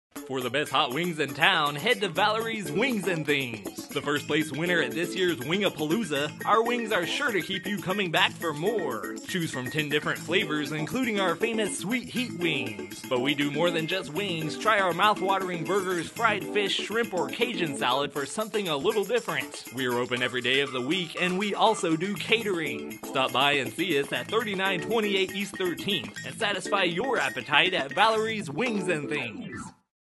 Valerie's Wings & Things
Commercial produced by Caleb Johnston at Great Plains Television for Valerie's Wings & Things in Wichita, KS.